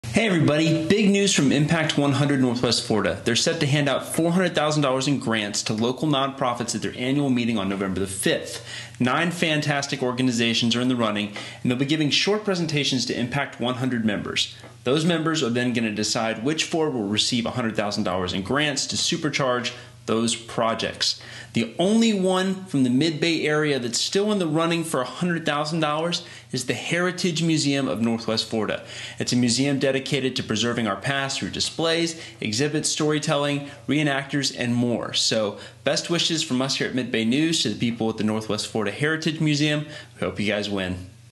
Impact 100 Grant Finalists Include One Valparaiso Non-Profit
🌟 Impact100 NW Florida is giving $400,000 in grants to local heroes, and one of our Valparaiso non-profits could be one of them! 🦸 Join us in celebrating community champions at their Annual Meeting.